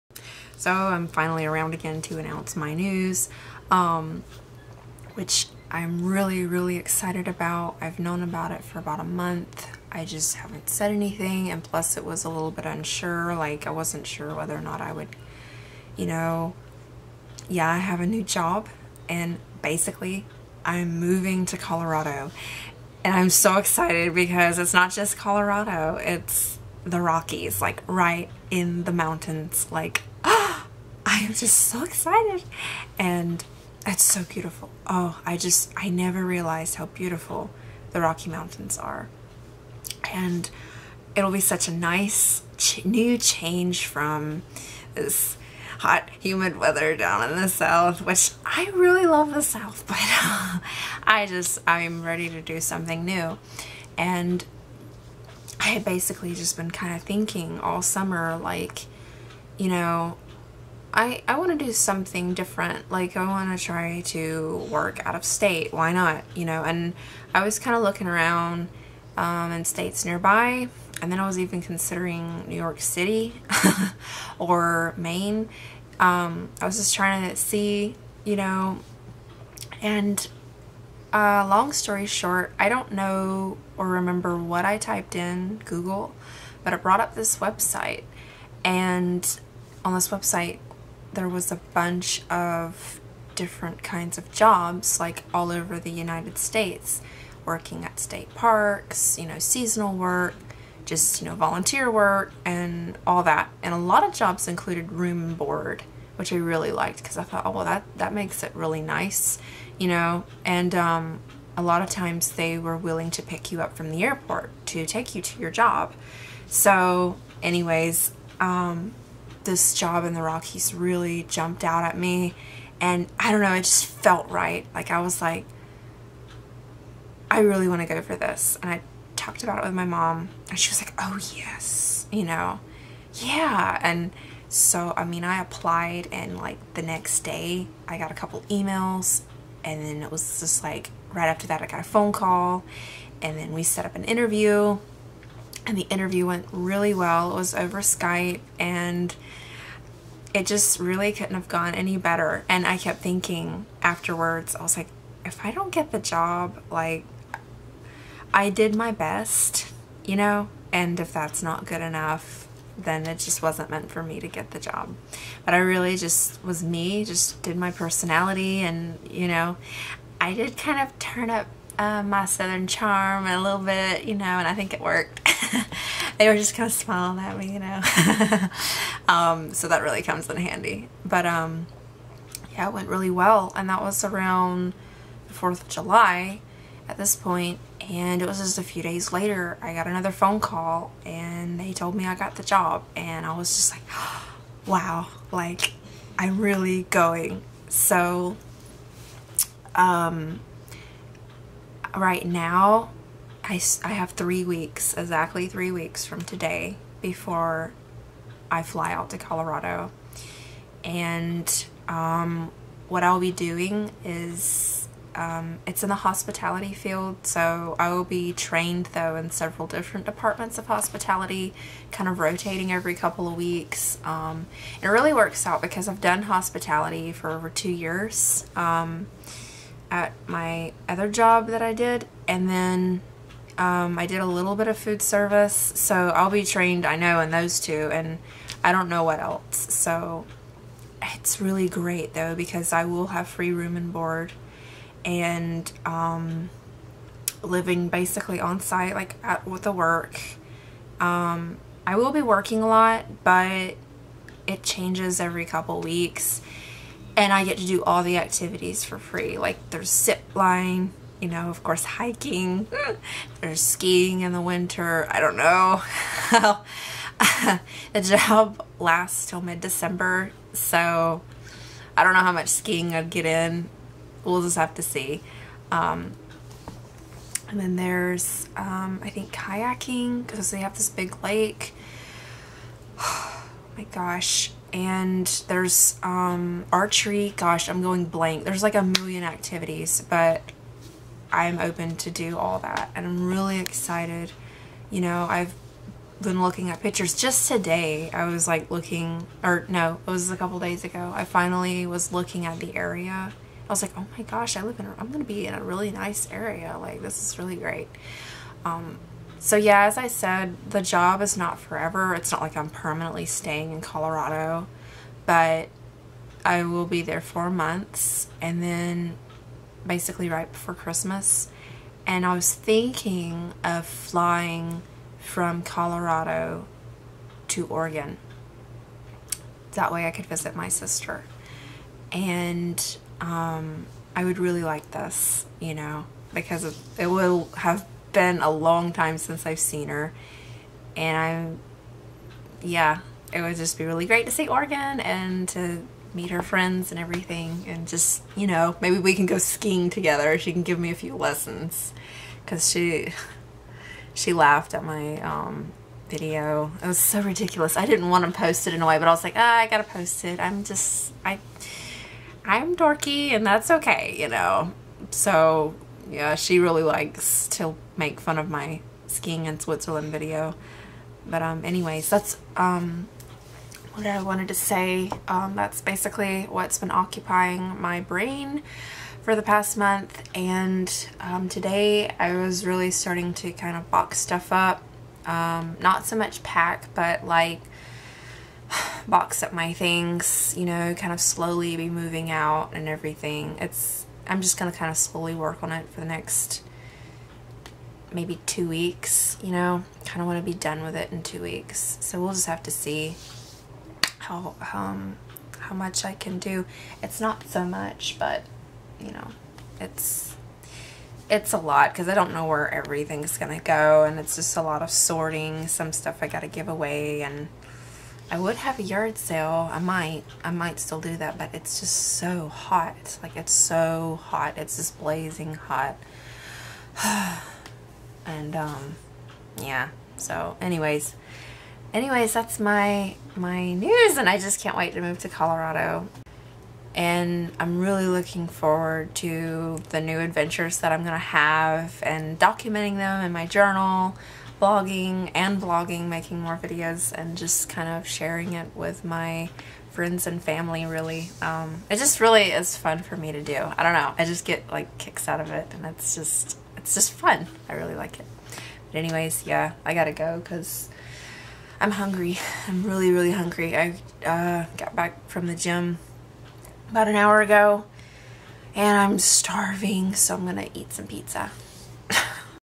(VLOG #44) Colorado here I come! 🌴✈🏔
I'll be moving out West to the Rockies next month - exactly 3 weeks from today! 😃 Counting down the days...
There's a lot for me to prepare for and get ready but I'm working on it :)
It's a bit of a change for me, but I'm ready to place myself in a different environment and be around new people and learning new things.

I've never been to Colorado before, much less the Rockies, and am excited to see what unfolds in the future😊😊